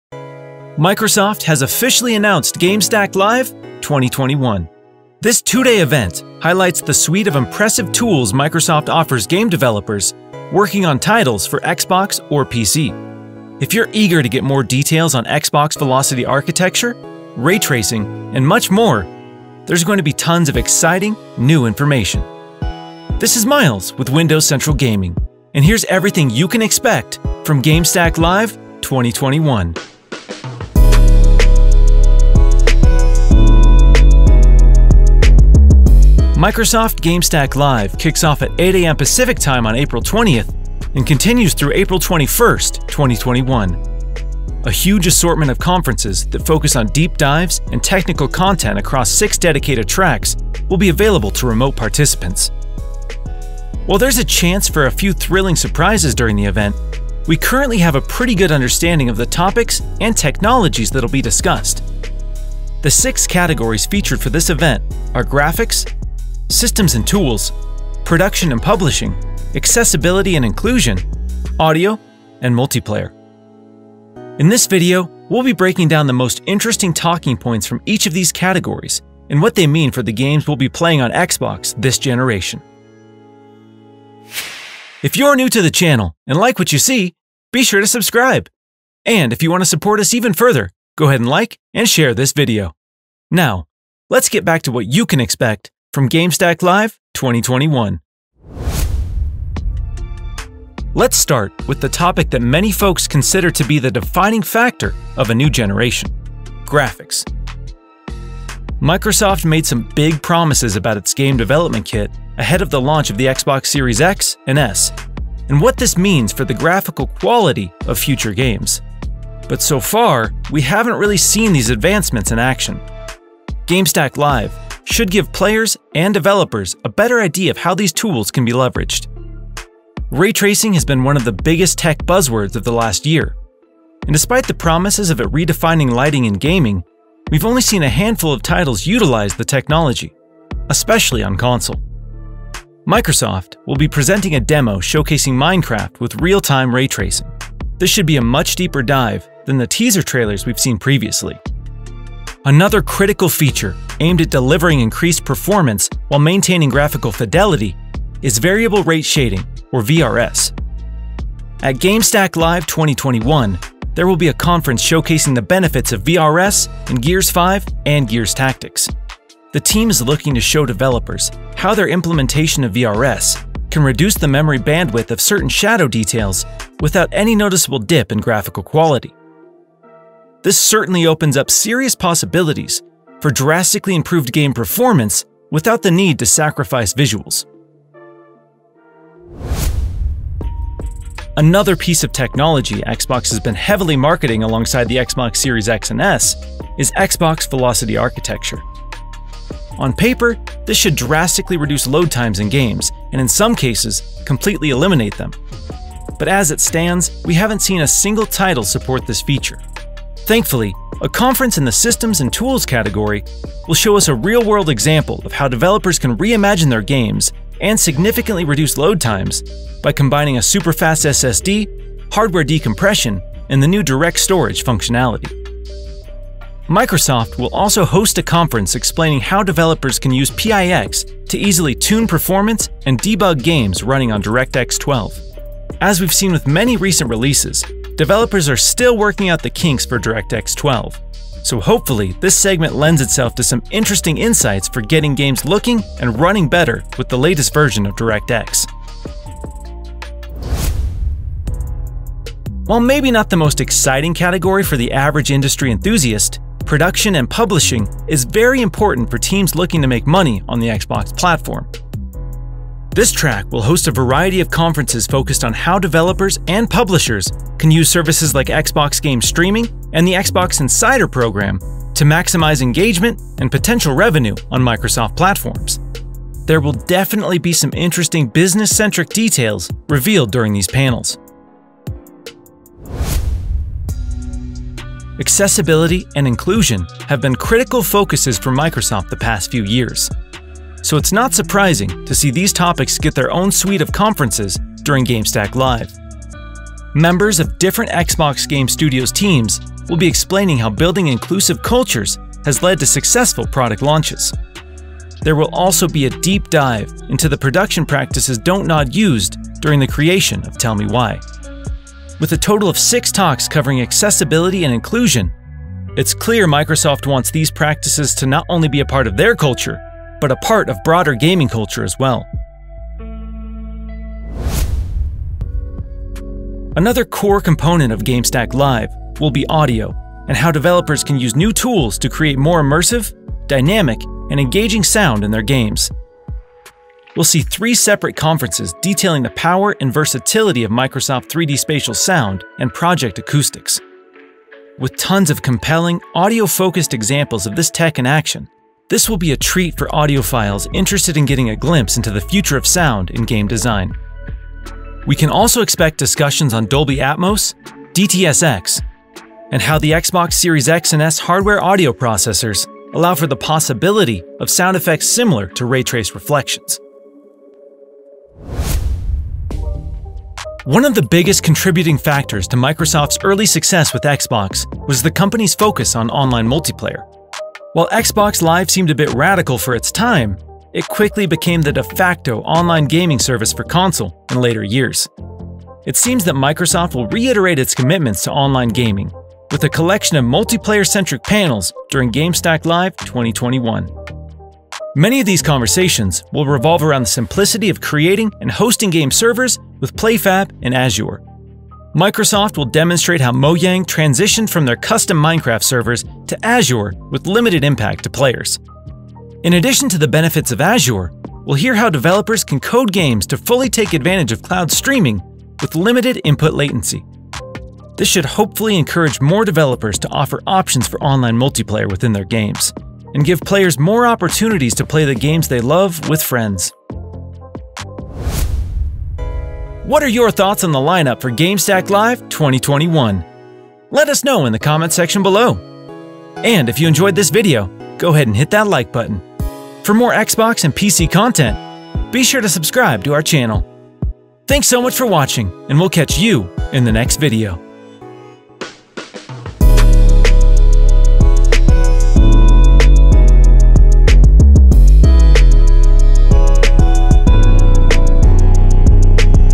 Game Stack Live 2021 - What to Expect from Xbox Series X & S with this Dev Event
Microsoft recently confirmed Game Stack Live 2021. This developer-focused showcase will feature deep dives into the tools available to teams making games for Xbox of PC. We've curated details on the most exciting panels coming with this event. Here's what you should expect when it comes to the future of the Xbox Series X and S game development.

Topics covered in this video:

Start - 00:00

When is Microsoft Game Stack Live 2021? - 00:37

Xbox Series X & S Graphics (VRS and Ray-Tracing) - 01:40

Xbox Velocity Architecture Demo - 03:15

Publishing on Xbox - 04:15

Xbox Accessibility and Inclusion - 04:48

Microsoft 3D Spatial Audio and Project Acoustics - 05:28

Xbox Multiplayer with PlayFab and Azure - 06:24

And to keep up to date with our gaming coverage outside of YouTube, catch us on the following socials:

Windows Central Gaming - @WCGamingTweets
Miles Dompier - @MilesDompier